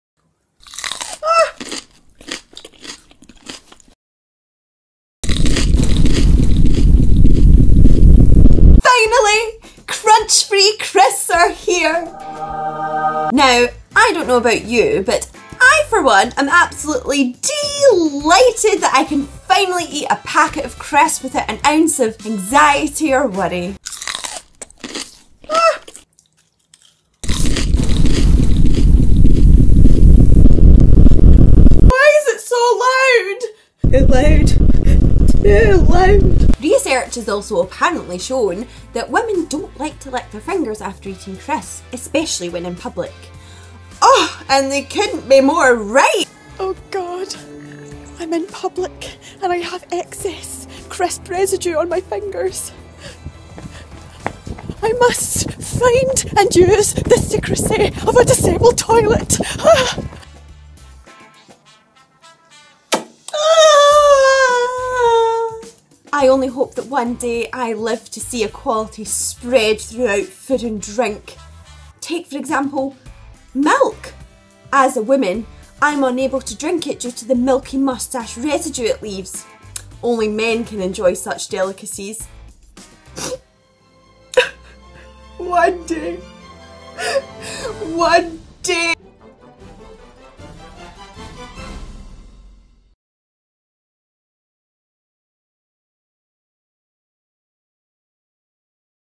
Crisps For Women?
Finally, crisps that women can eat without fear of the loud crunching noise!

In this video, Natalie is thrilled to hear the news about crisps for women.